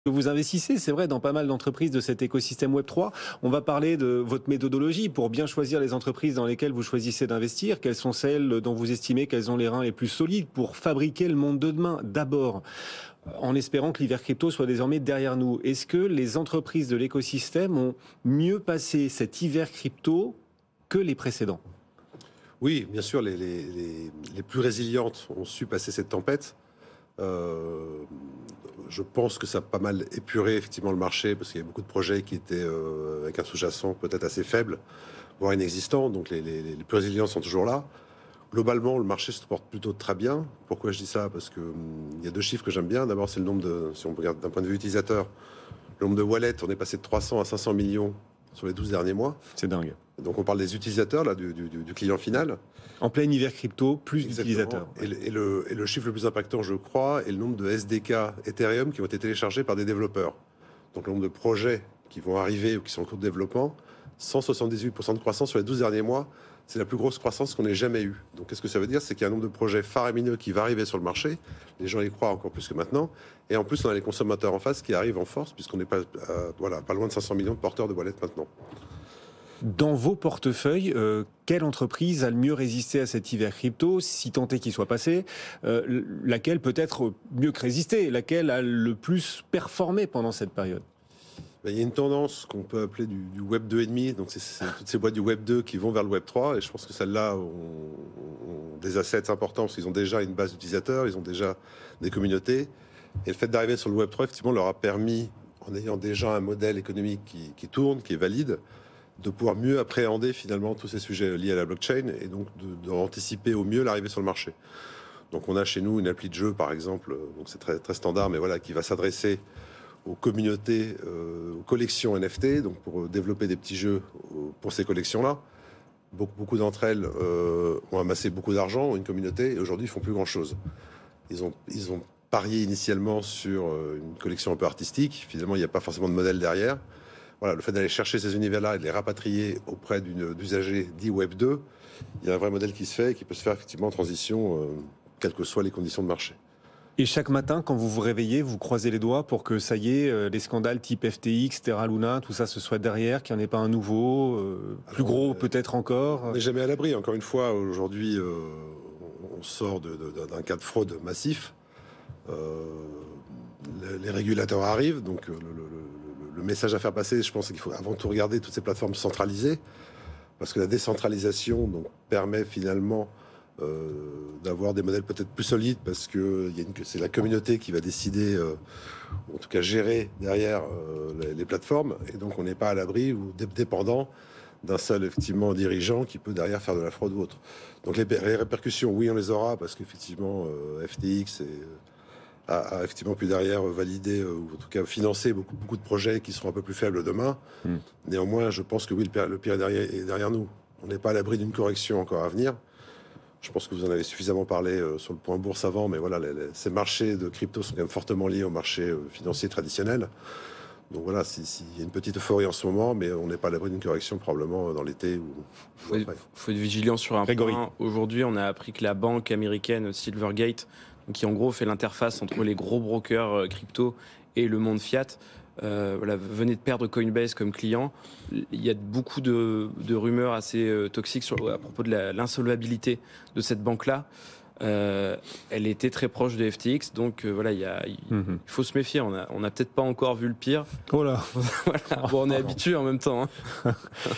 Crypto : quelles sont les dynamiques d'investissement ?
- Quelle entreprise web 3 a le moins souffert durant l'hiver crypto ?
- Les scandales FTX et Terra-Luna sont-ils derrière nous ?

Gonzague de la Tournelle, conseiller du fonds w3i.fund